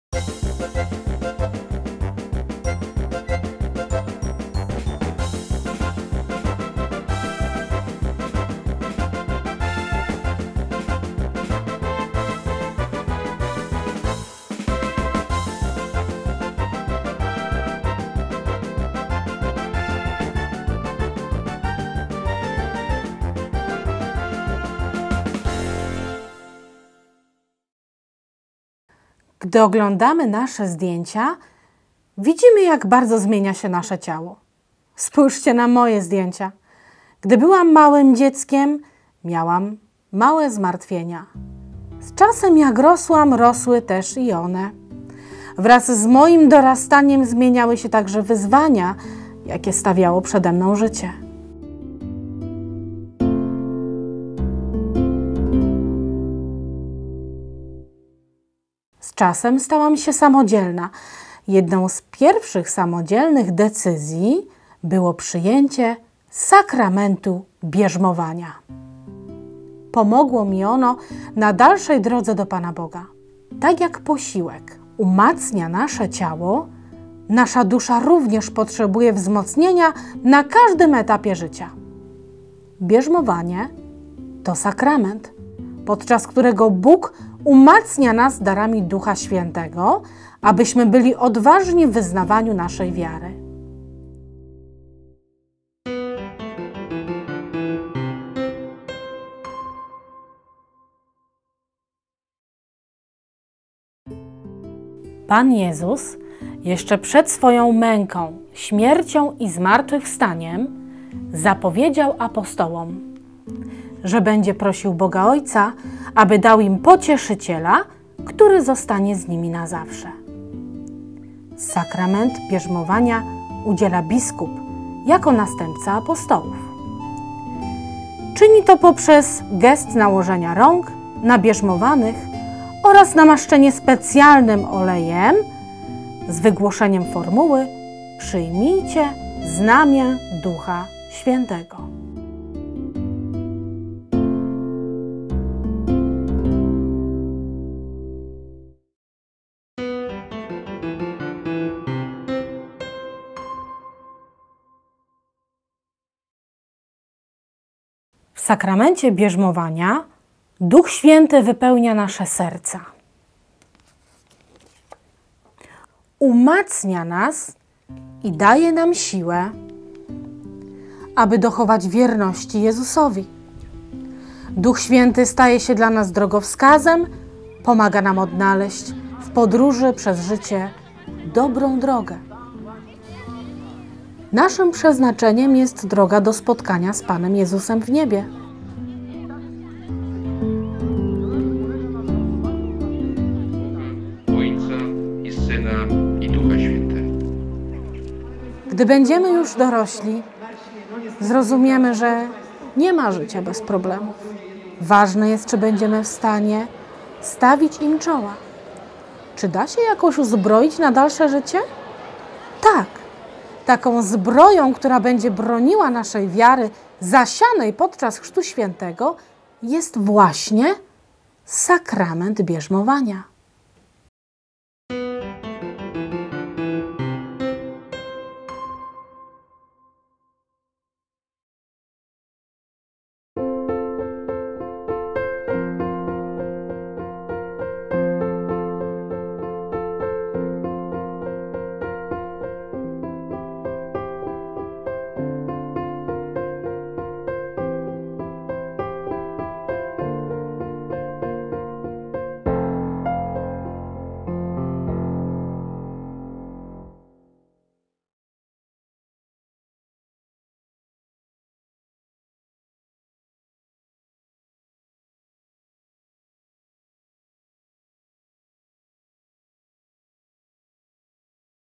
Katecheza SM@RT bierzmowanie
Sakramenty są darami Pana Jezusa. Dzięki nim jesteśmy włączeni w Tajemnicę życia, śmierci, męki i zmartwychwstania Zbawiciela oraz stajemy się członkami Mistycznego Ciała Chrystusa jakim jest Kościół. Sakramenty są to widzialne znaki niewidzialnej łaski. Sakramenty mają swój głęboki wymiar duchowy. W krótkich katechezach dla najmłodszych dzieci prezentujemy poszczególne sakramenty.  Bierzmowanie jest sakramentem, w którym otrzymujemy w szczególny sposób Ducha Świętego. Obok Chrztu i Eucharystii należy do Sakramentów Wtajemniczenia Chrześcijańskiego. Jest to Sakrament, w którym Duch Święty umacnia człowieka udzielając mu siedmiorakich darów mających pomóc młodemu człowiekowi w mężnym wyznawaniu swojej wiary i postępowaniu według jej zasad.